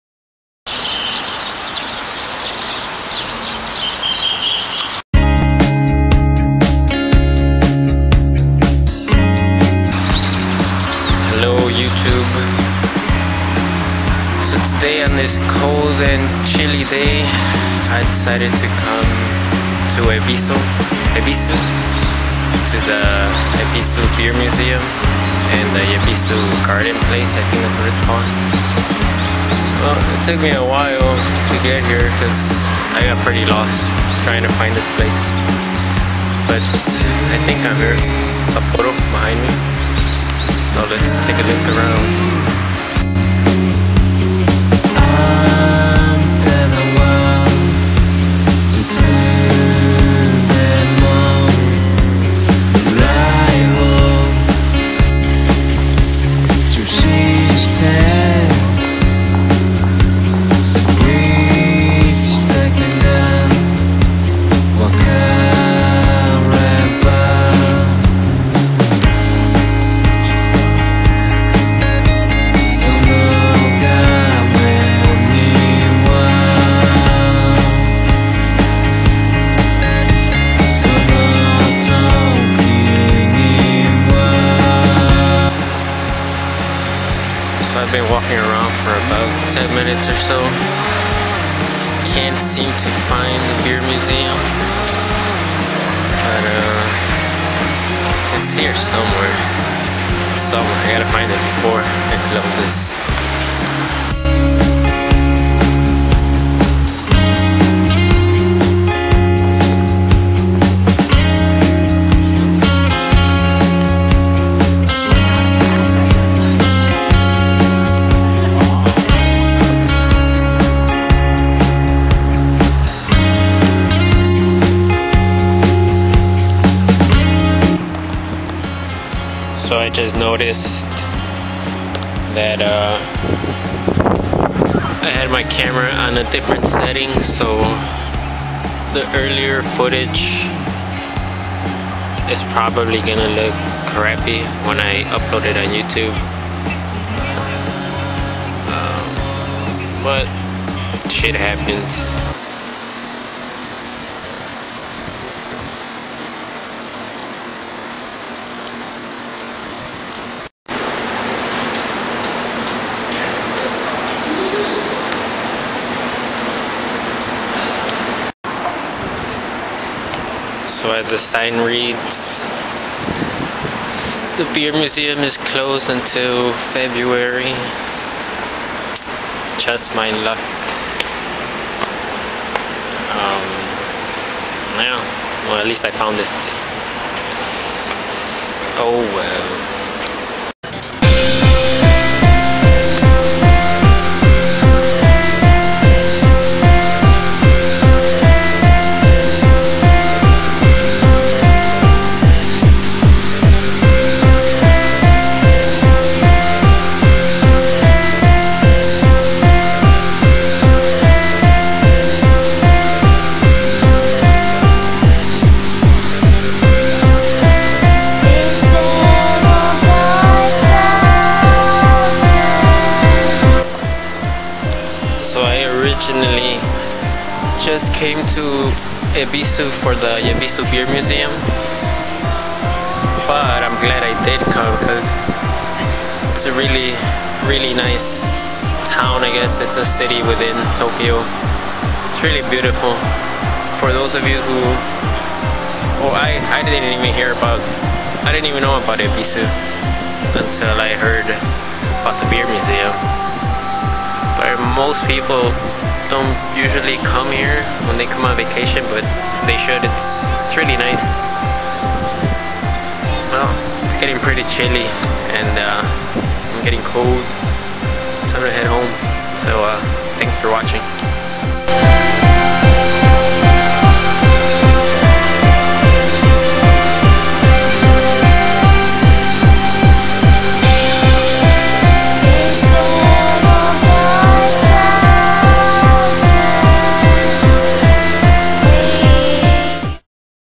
Tony in Japan #11 : Ebisu & Yebisu Garden Place
Hey guys! So this footage is from about a couple of weeks before Christmas, so it is pretty old. I went to Ebisu and checked out Yebisu Garden Place. I also saw the Beer Museum, which was awesome!! For about the first minute and a half of the video, the quality is not as good because I had my camera on a different setting... Well enjoy!

**Old Footage**